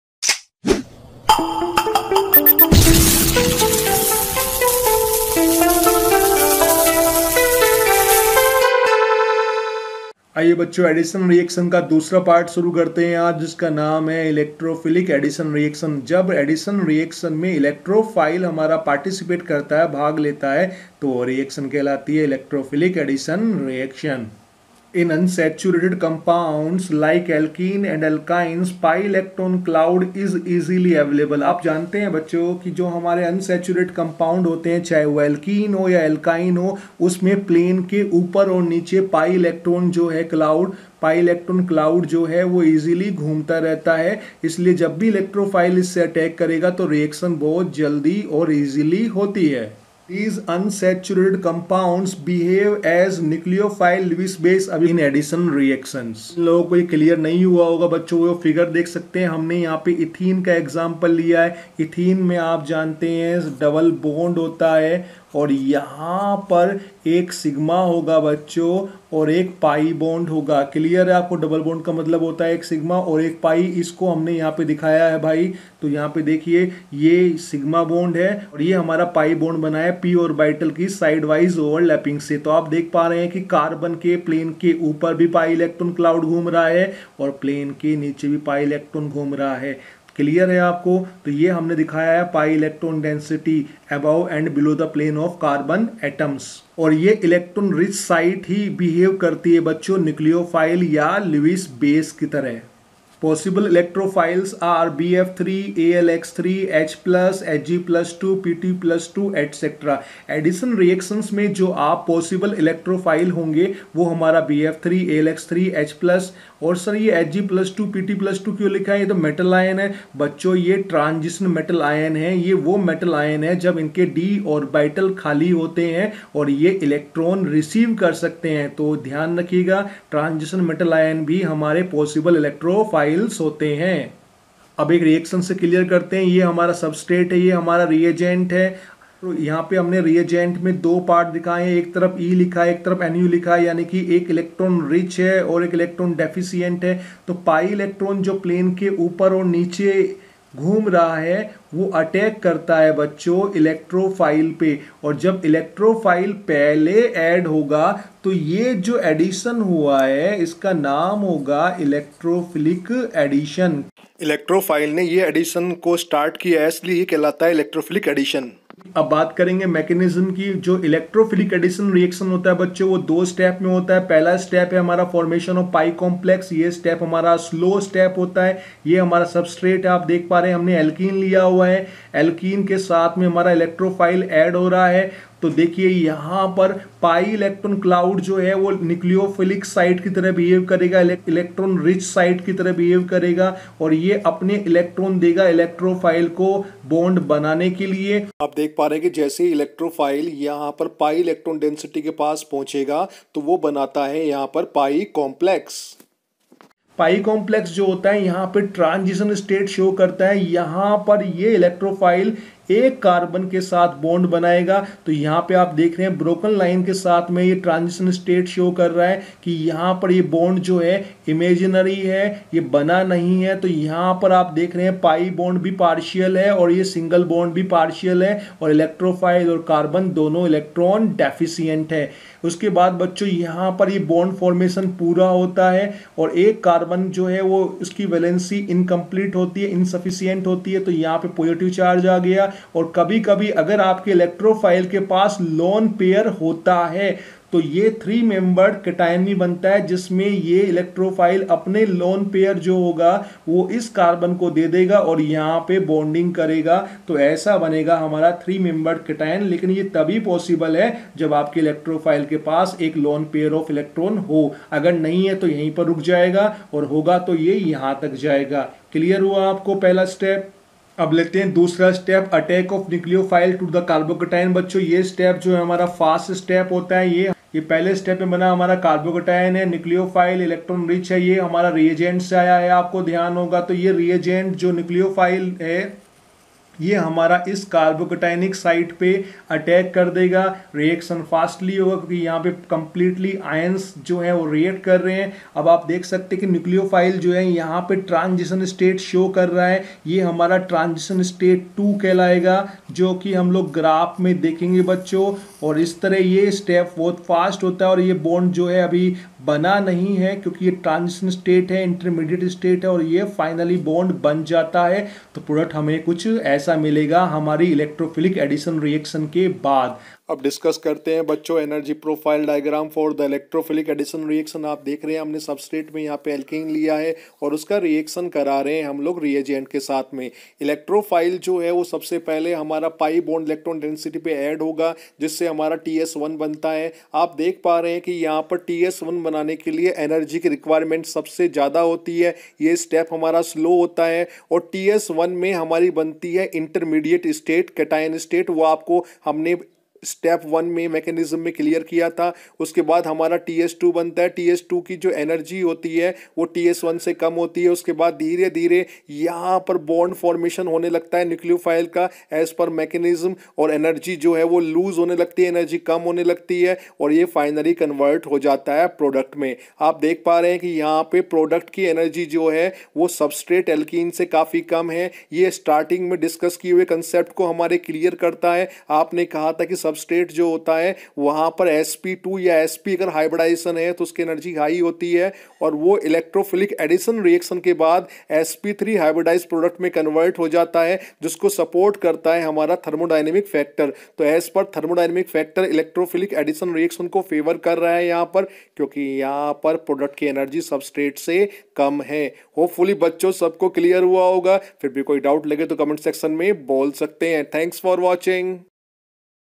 Electrophilic Addition Reaction, Part# 2, Chemistry Junction
Characteristics, mechanism and energy profile diagram explained here.
Chemistry Junction